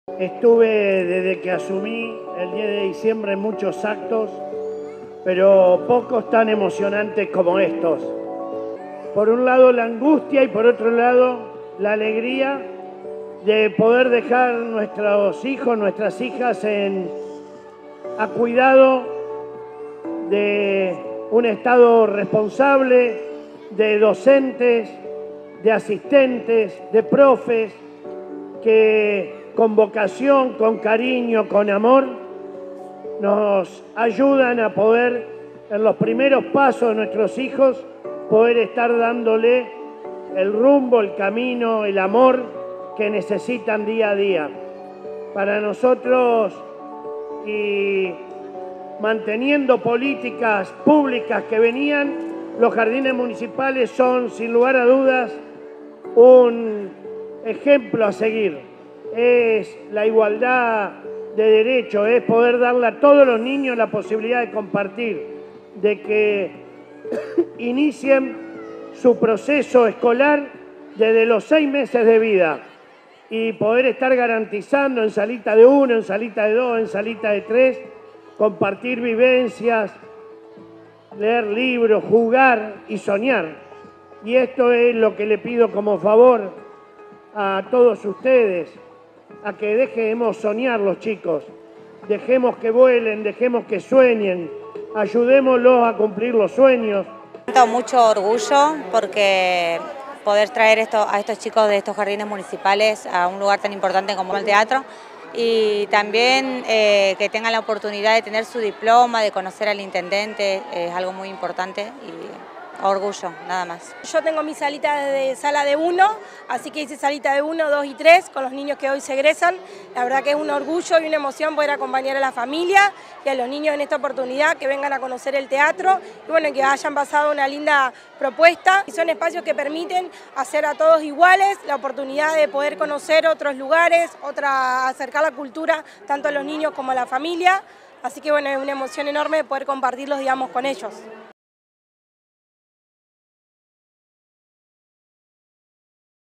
Jardines Municipales: 650 alumnos y alumnas egresaron de las salas de 3 años
En el Teatro Municipal, los niños y niñas de los 17 espacios municipales recibieron sus diplomas y recordatorios. El intendente Juan Pablo Poletti aseguró que “estos espacios son un ejemplo a seguir”; y agradeció la labor que directivos y docentes realizan a diario, y a las familias por el acompañamiento y el compromiso.